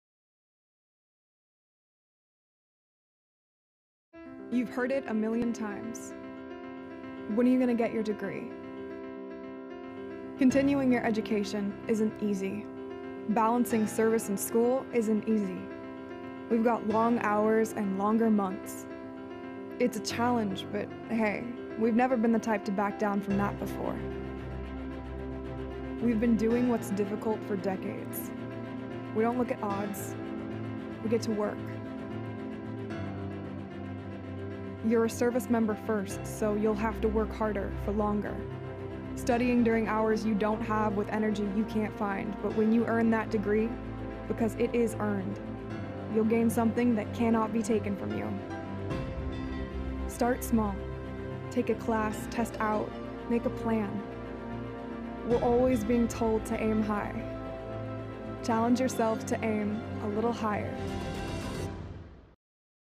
Service and School
Credit: Airman 1st Class Kirsten Brandes | Date Taken: 06/06/2017
A social media video discussing the difficulty of pursuing higher education while serving in the military for National Higher Education Day.